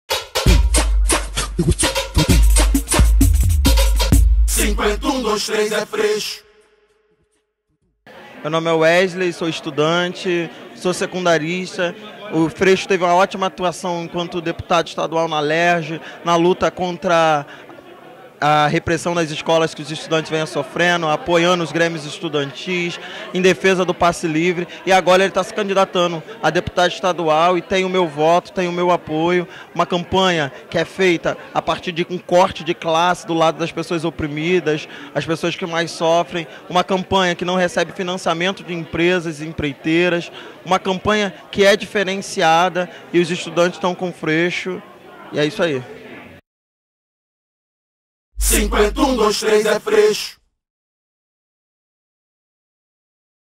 Wesley: "Por que fecho com Freixo?"
Wesley, estudante, declara seu voto em Marcelo Freixo.